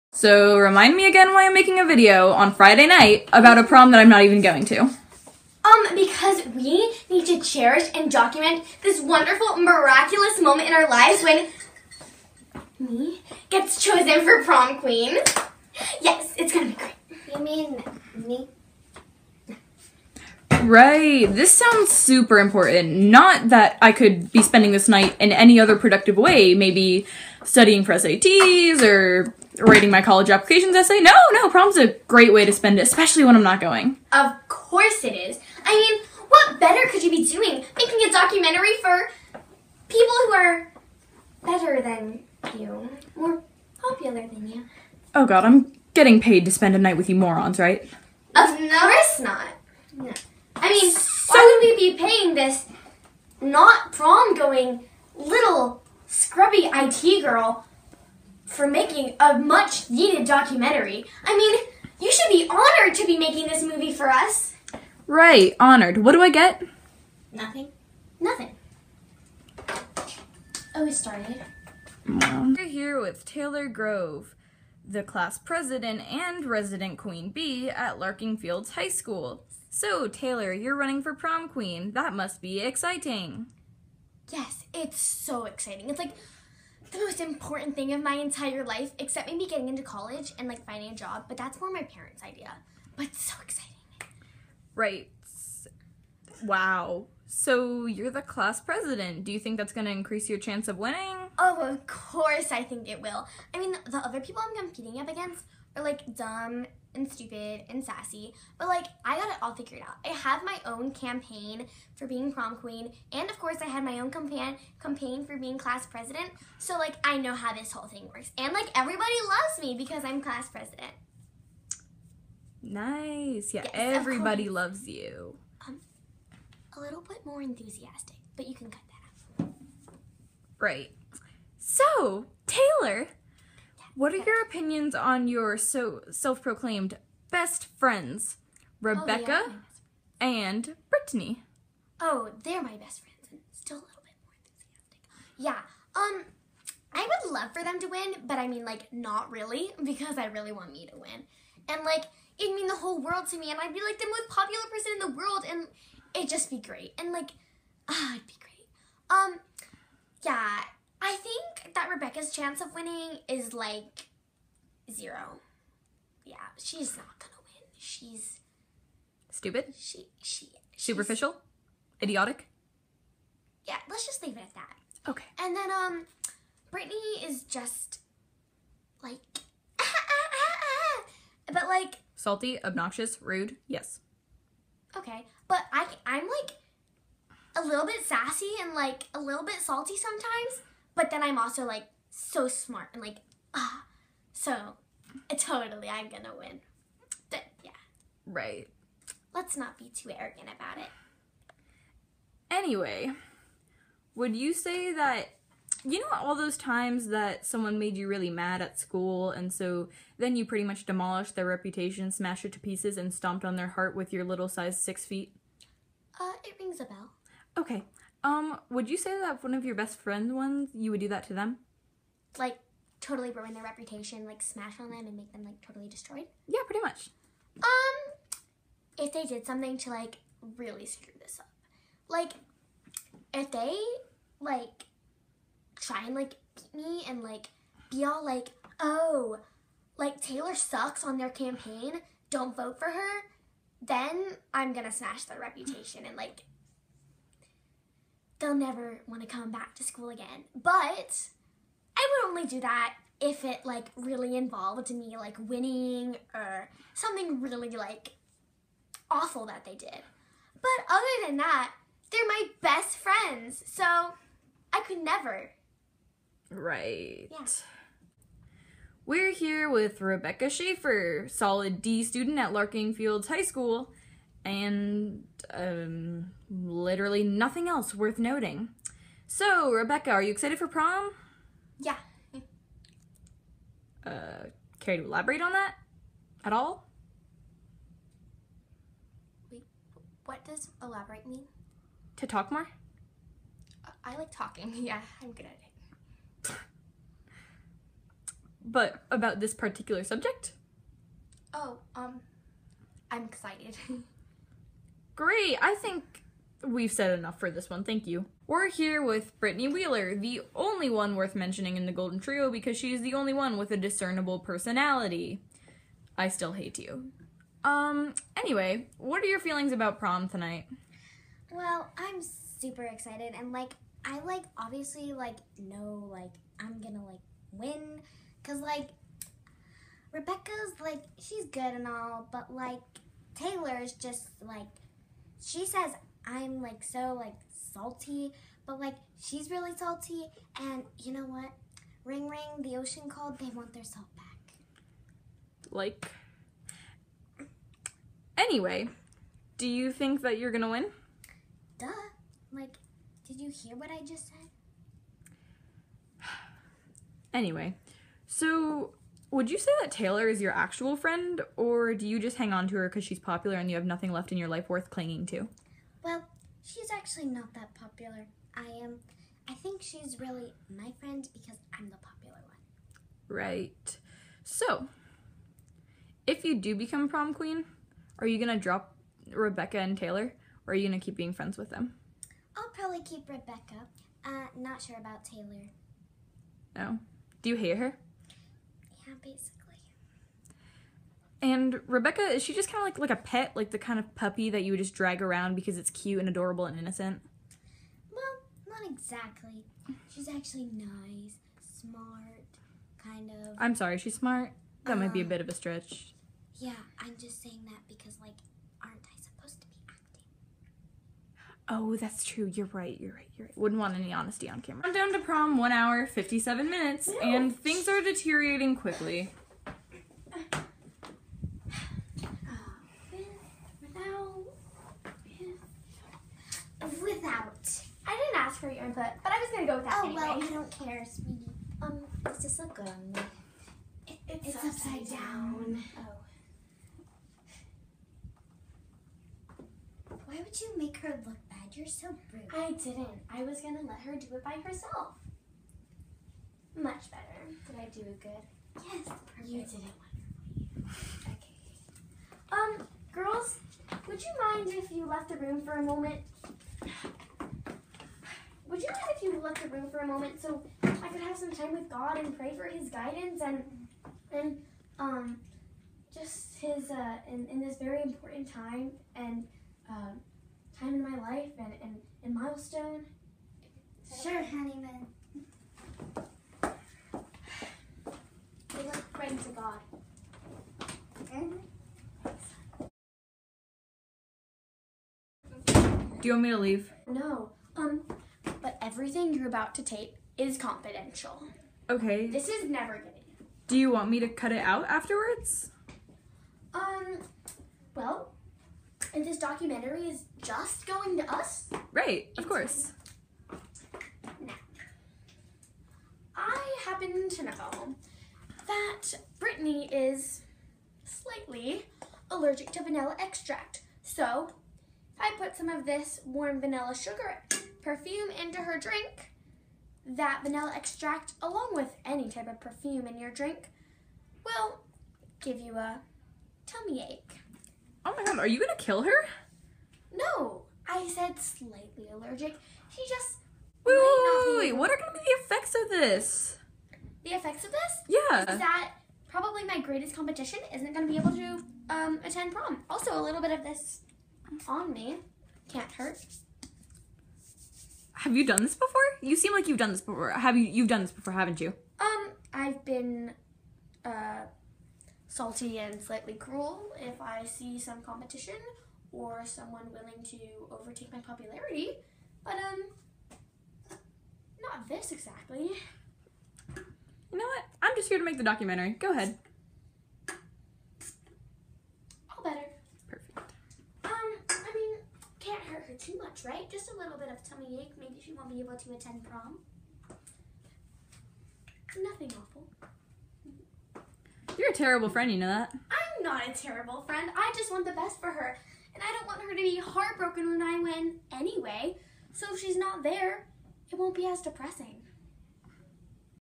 Drama Queens (Part 1)
This is movie is about a nerd teenage girl who makes a documentary on three selfish teenage divas from her school who are convinced they will make prom queen. However, things can't go perfectly as planned...
Sorry the Part 1 had come a lot after the Part 2. It had trouble uploading.